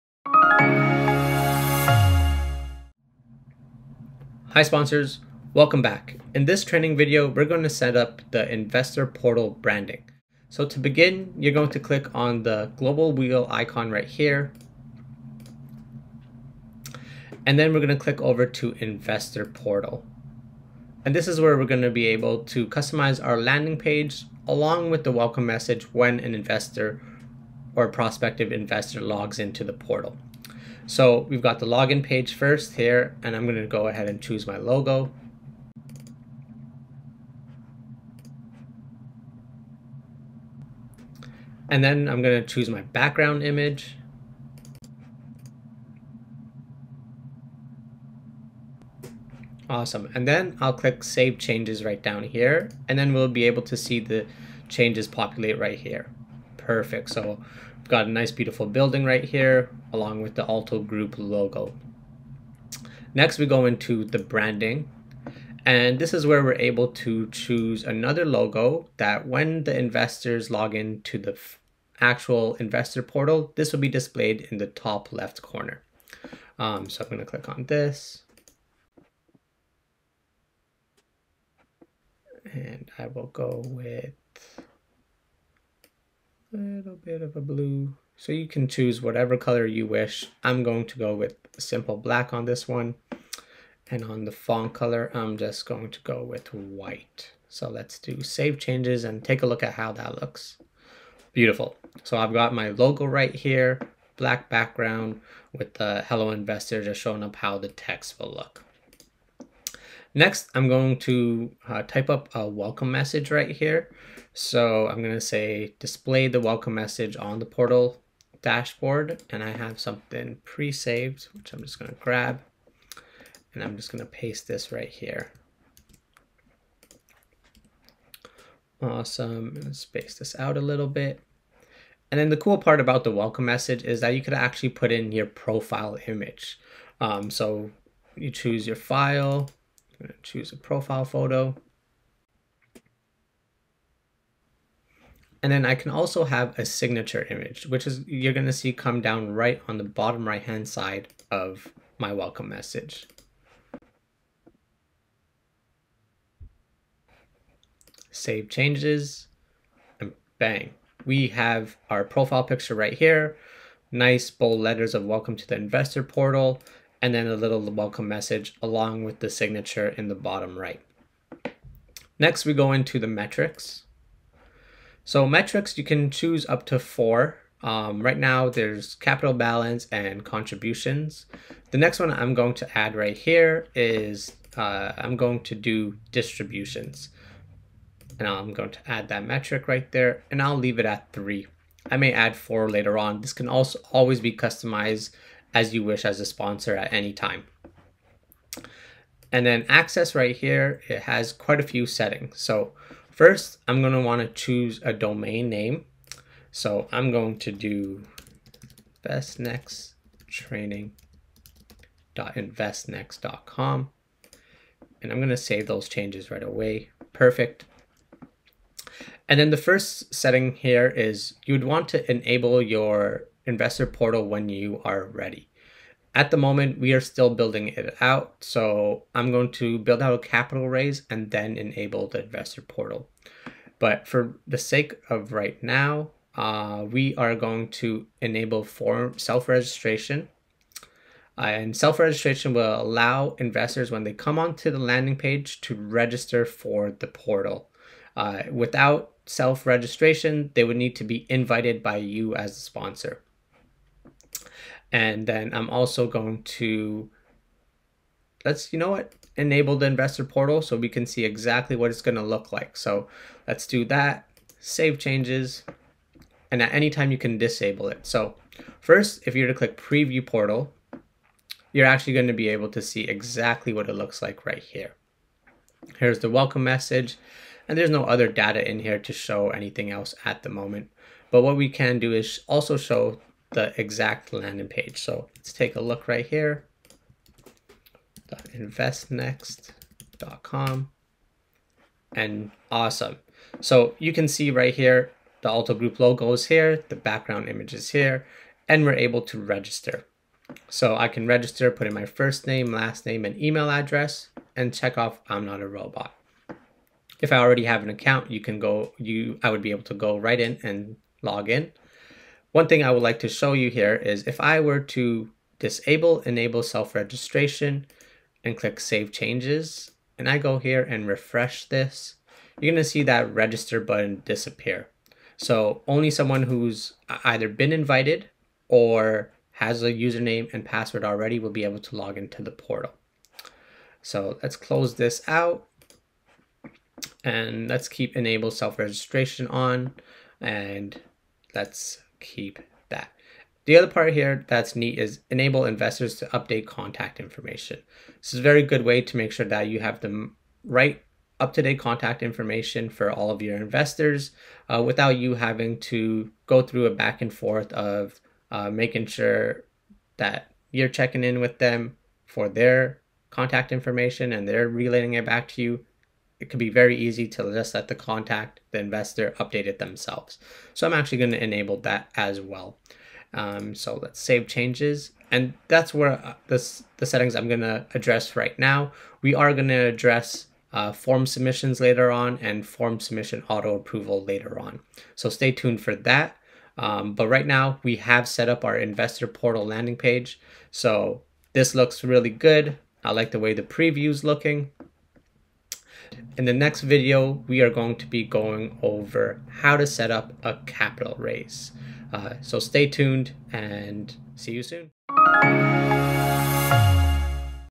Investor Portal Set Up - Onboarding
A guide through setting up your Investor Portal branding with your logos, a welcome message, metrics to share with your investors, and setting up your subdomain.

0:22 → Customize the Investor Portal Login Page & Branding
1:50 → Customize the Welcome Message
2:54 → Select Metrics to display on the Investor Portal
3:24 → Set your subdomain, Enabling your Investor Portal